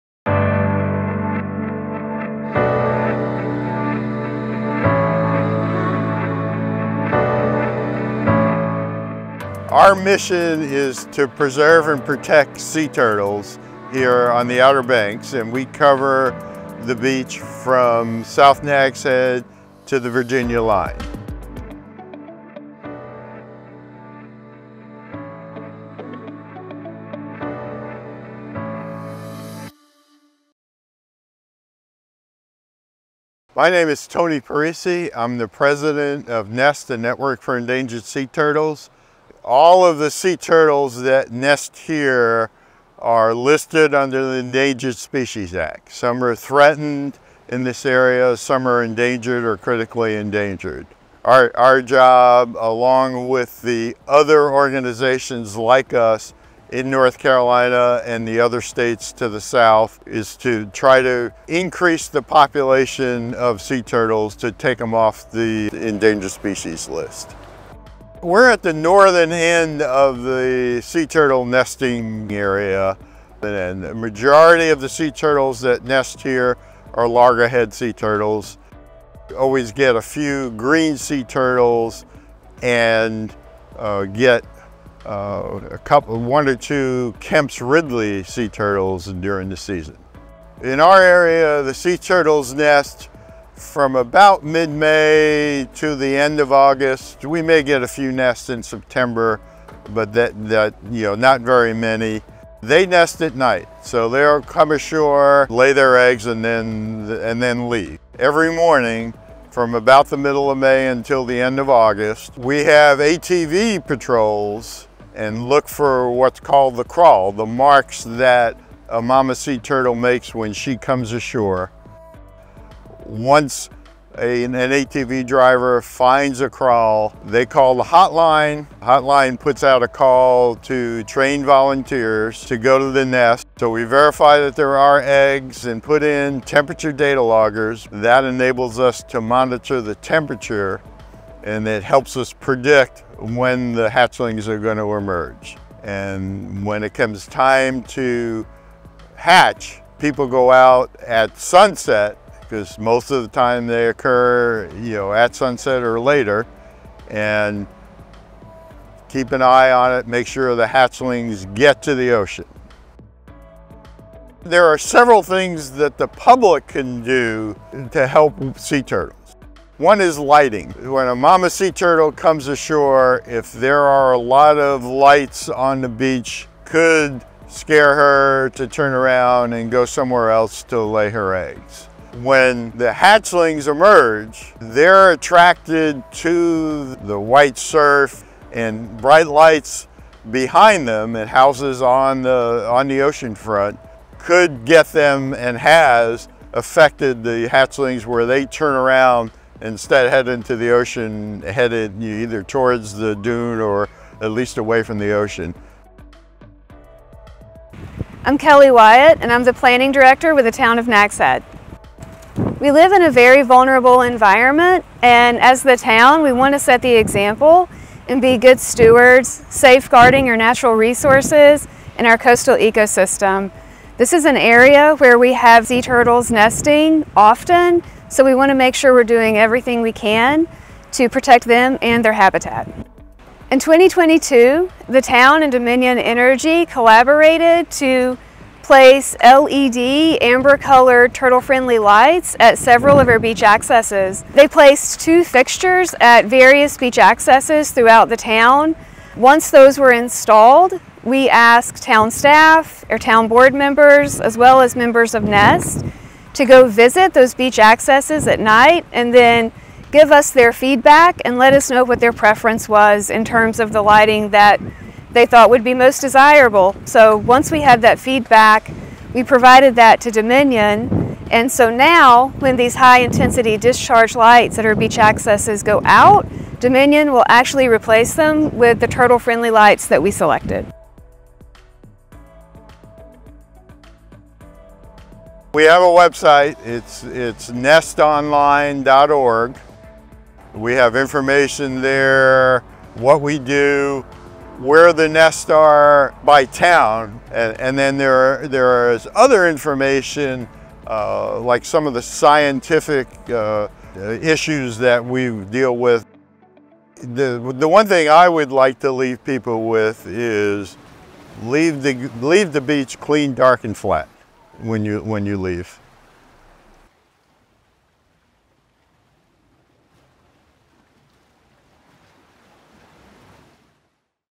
Journey of the Sea Turtles - Nurturing Nature in Nags Head
Learn about the great work that N.E.S.T. (Network for Endangered Sea Turtles) and the Town of Nags Head, NC is doing to help endangered Sea Turtles. 👏